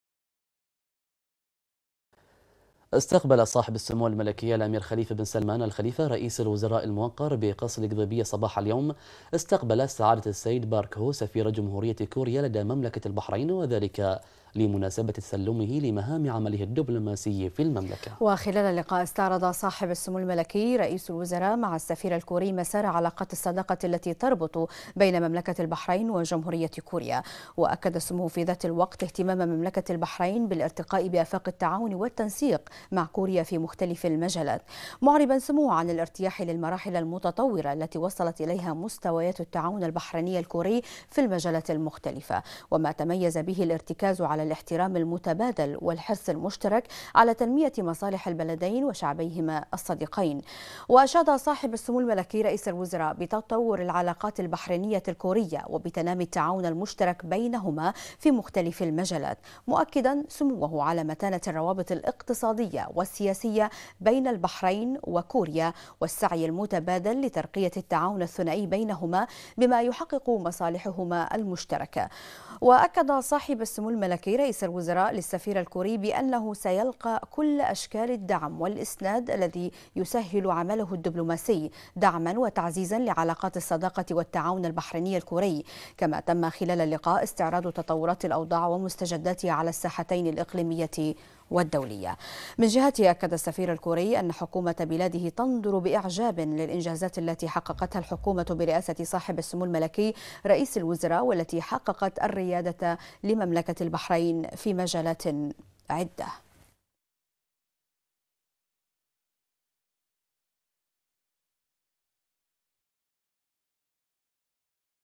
البحرين: سمو رئيس الوزراء يستقبل سفير جمهورية كوريا لدى المملكة
المنامة في 7 سبتمبر / بنا / استقبل صاحب السمو الملكي الأمير خليفة بن سلمان آل خليفة رئيس الوزراء الموقر بقصر القضيبية صباح اليوم سعادة السيد بارك هو سفير جمهورية كوريا لدى مملكة البحرين، وذلك بمناسبة تسلمه مهام عمله الدبلوماسي في المملكة.